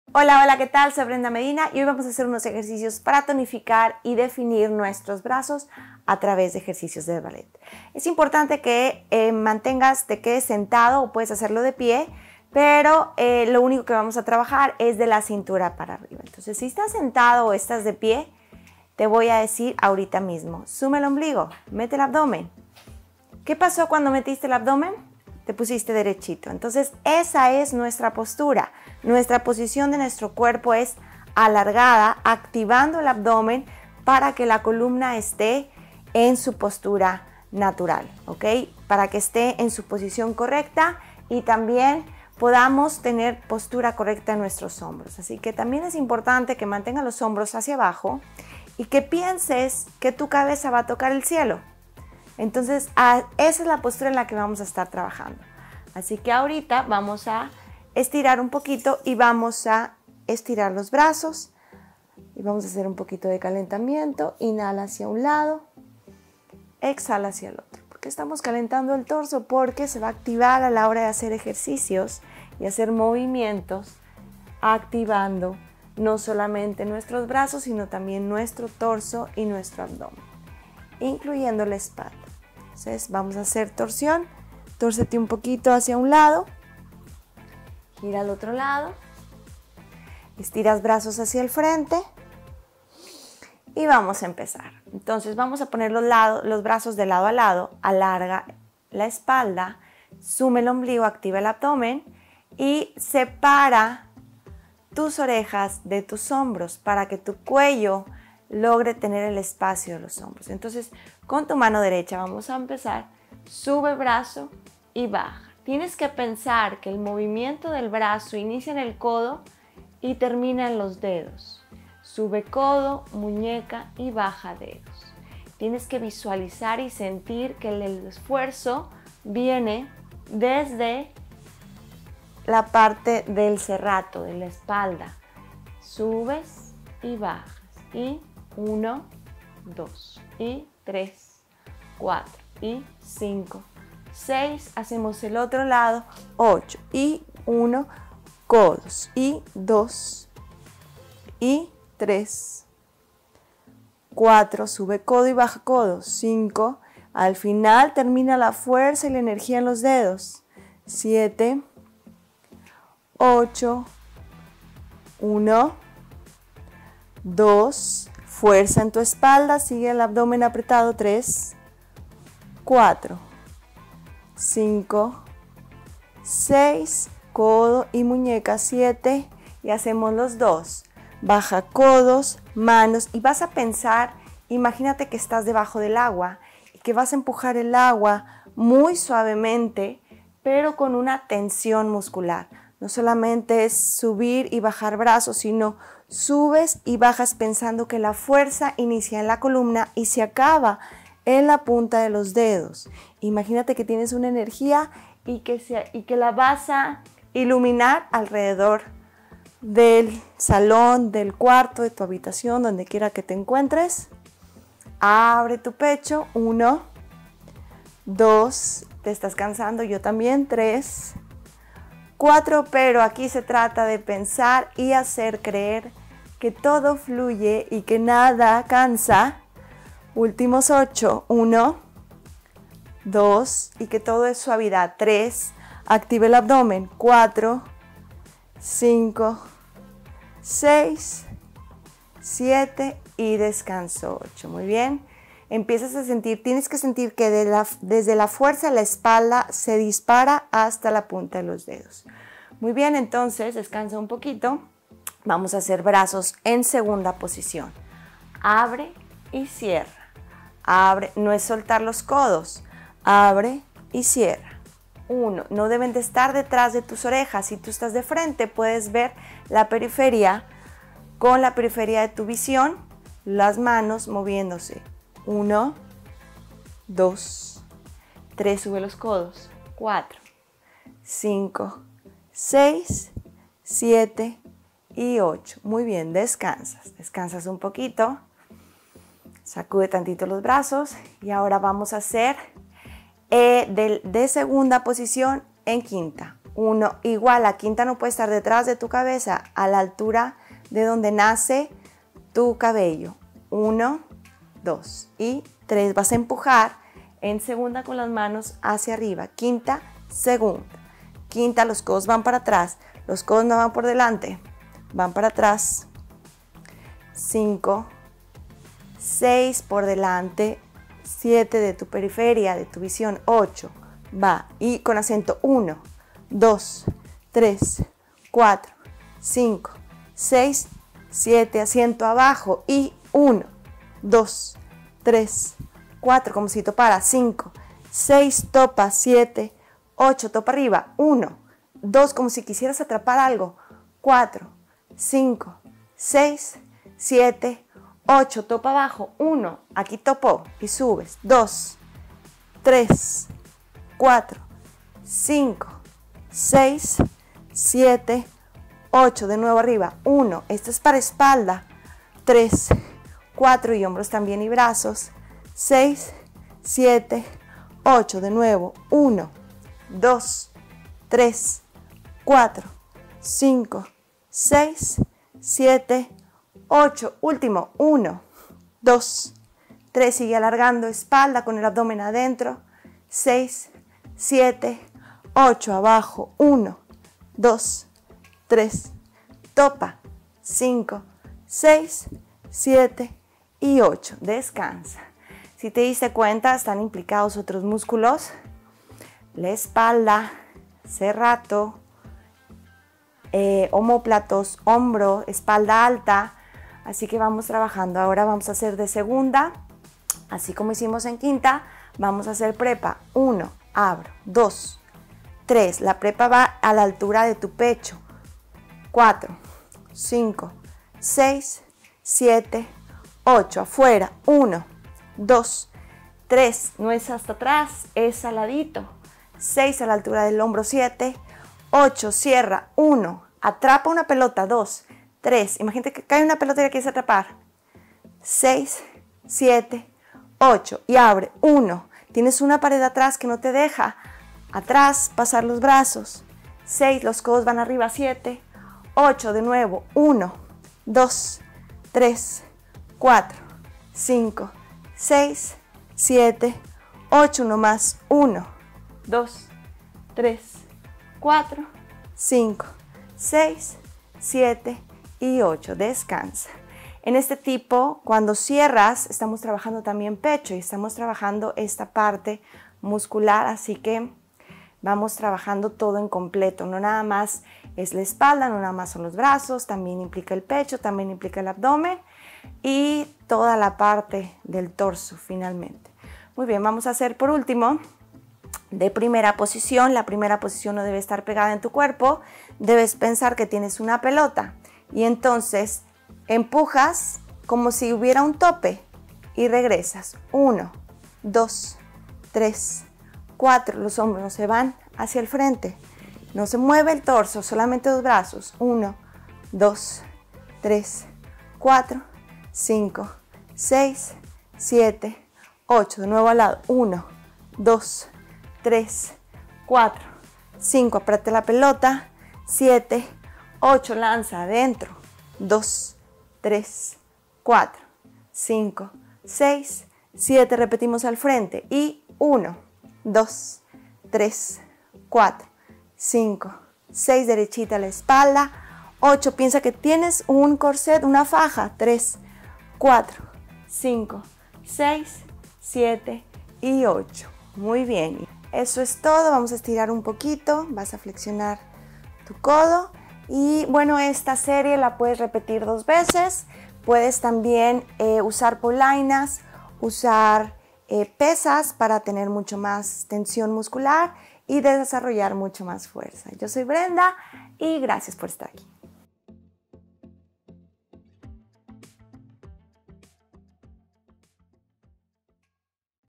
¡Ejercitemos nuestros brazos juntas! Rutina de Brenda Medina
Hola a todas, aquí les va una super rutina de Brenda Medina para ejercitar y definir nuestros brazos. Es super buena y la podemos hacer en cualquier lugar de nuestra casa.

¡Hagámoslo juntas!